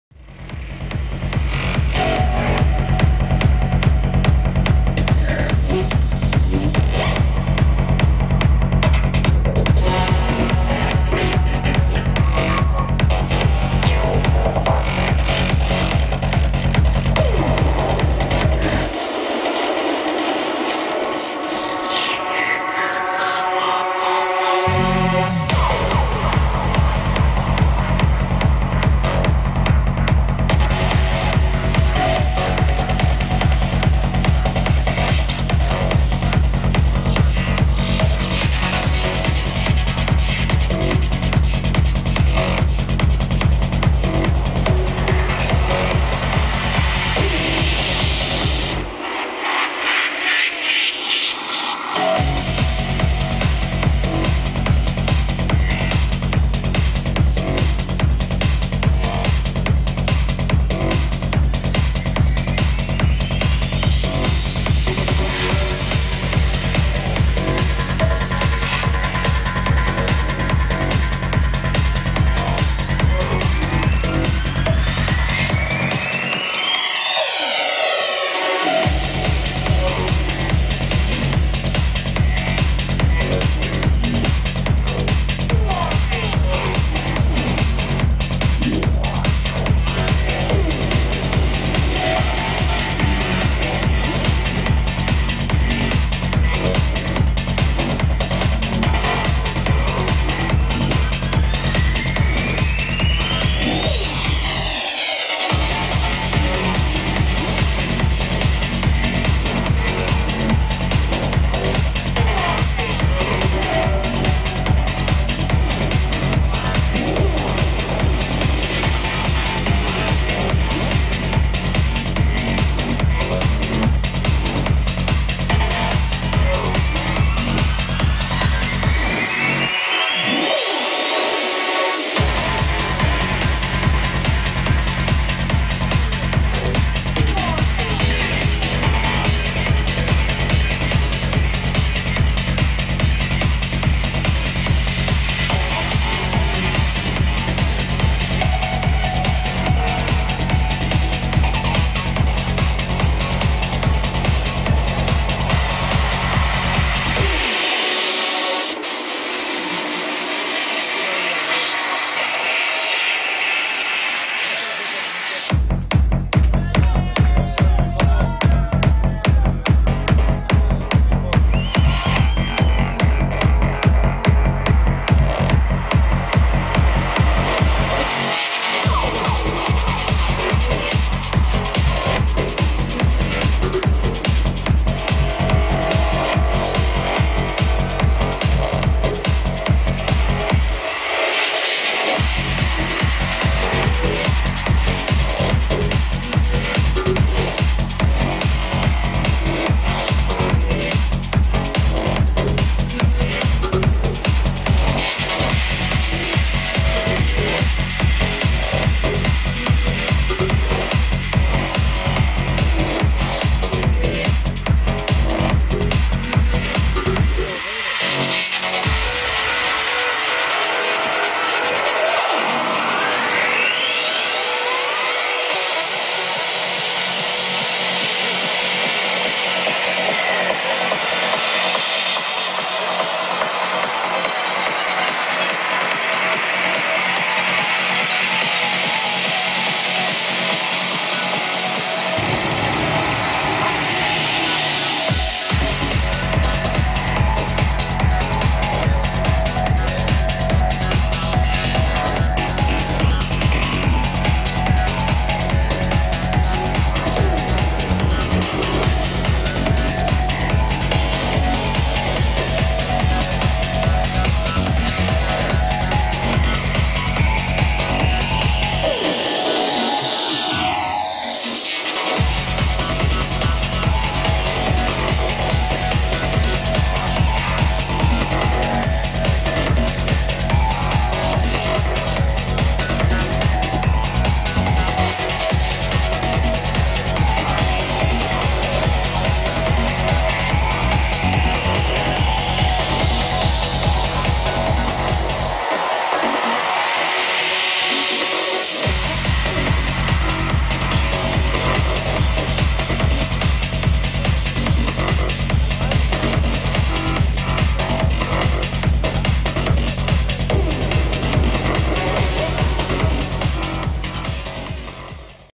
Soraswatt @ Infected Guitars Mini Festival (Space Music Drops) Full HD
Evento: Infected Guitars Mini Festival  (09 e 10 /07/2016)
Por: Space Music  Drops
Local: Corujeira, Mira Portugal